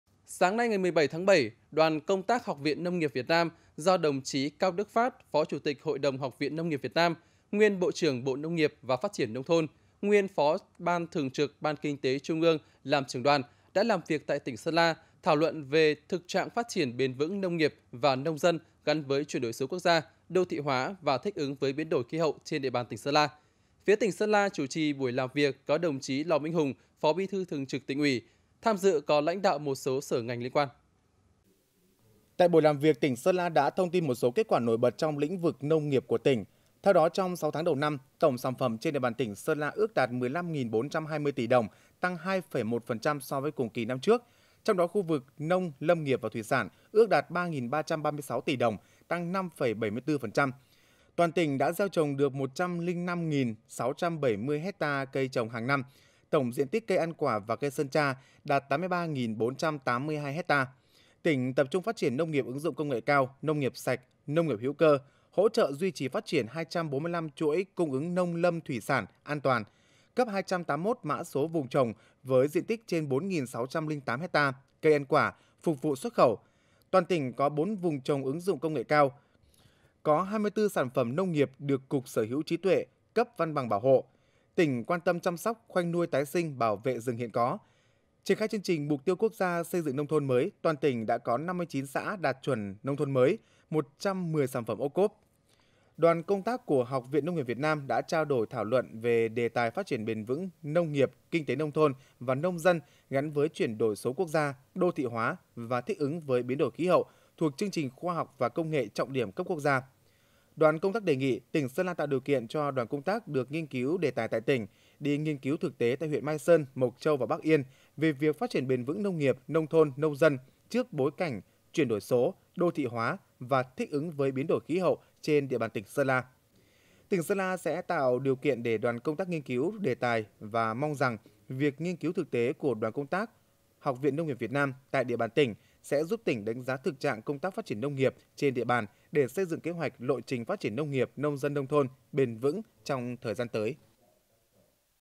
Đoàn công tác Học viện Nông nghiệp Việt Nam làm việc tại tỉnh Sơn La
Sáng ngày 17/7, Đoàn công tác Học viện Nông nghiệp Việt Nam do đồng chí Cao Đức Phát - Phó Chủ tịch Hội đồng Học viện Nông nghiệp Việt Nam, nguyên Bộ trưởng Bộ Nông nghiệp và Phát triển nông thôn, nguyên Phó Ban thường trực Ban Kinh tế Trung ương làm trưởng đoàn đã làm việc tại tỉnh Sơn La thảo luận về thực trạng phát triển bền vững nông nghiệp và nông dân gắn với chuyển đổi số quốc gia, đô thị hóa và thích ứng với biến đổi khí hậu trên địa bàn tỉnh Sơn La. Phía tỉnh Sơn La, chủ trì buổi làm việc có đồng chí Lò Minh Hùng - Phó Bí thư Thường trực Tỉnh ủy. Tham dự có lãnh đạo một số sở, ngành liên quan.